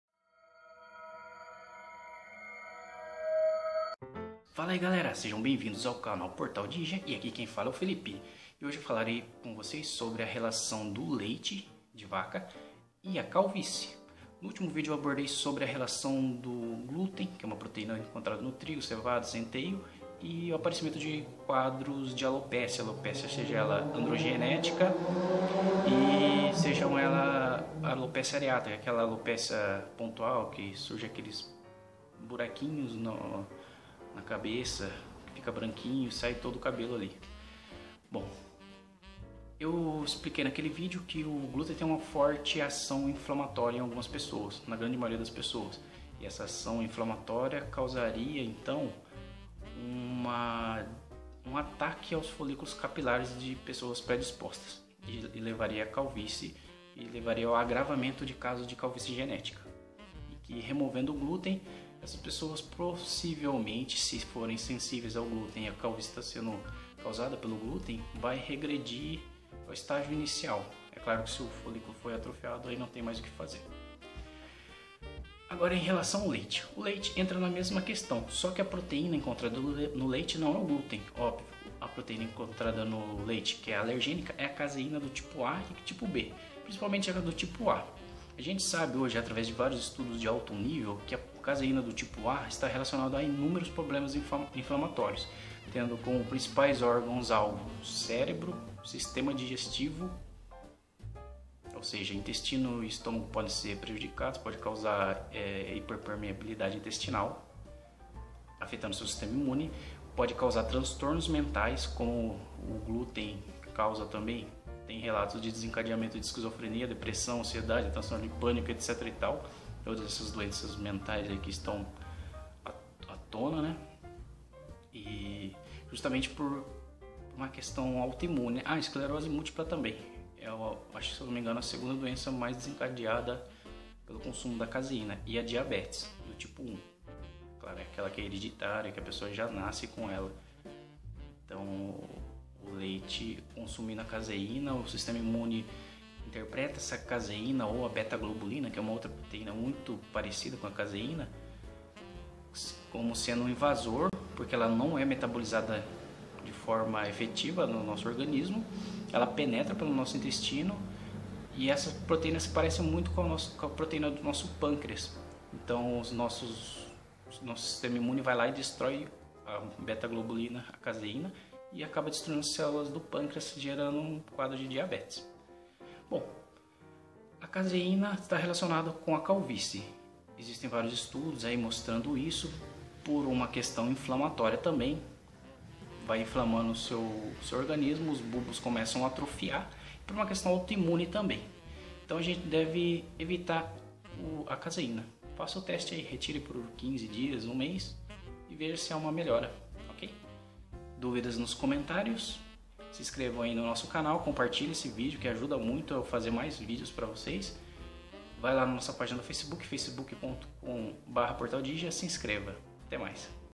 LEITE E CALVÍCIE - LEITE CAUSA CALVÍCIE?
Breve vídeo comentando se existe a relação entre o consumo de leite e surgimento de quadros de calvície.
Lembrando que a unica calvície realmente relacionada com a alergia ao leite é a AREATA(universal), porém a alergia à(s) proteína(s) do leite(Caseína A/B // α-β-Globulina) pode levar a inflamação cronica de todas as células do corpo, incluindo os folículos capilares, podendo agravar um quadro de alopécia androgênica.

Bibliografia para consulta posterior:
Sintomas das alergias alimentares: